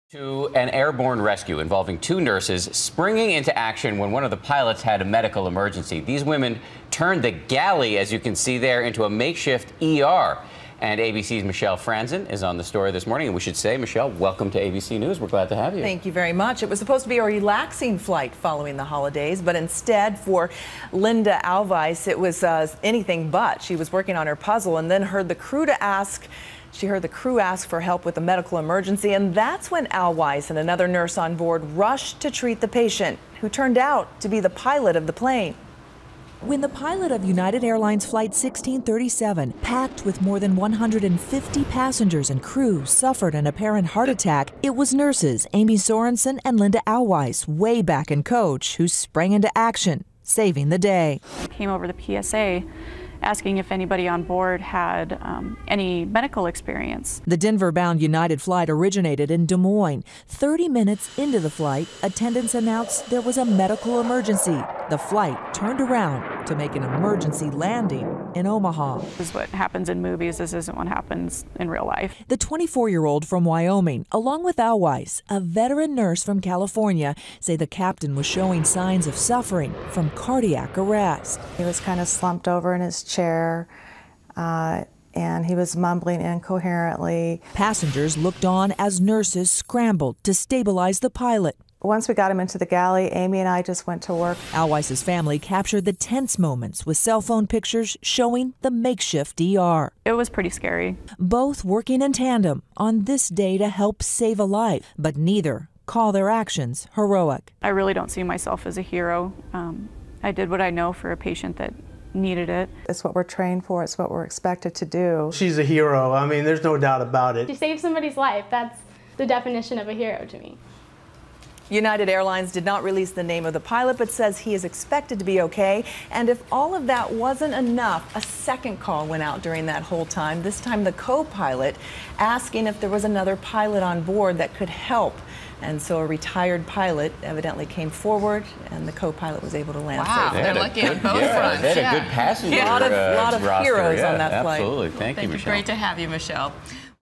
Nurse Saves Pilot's Life in Midair Plane Galley Turned Into Emergency Room Midflight United Airlines
United Airlines Pilot receives life-saving care after flight crew finds passenger that has the skills to help.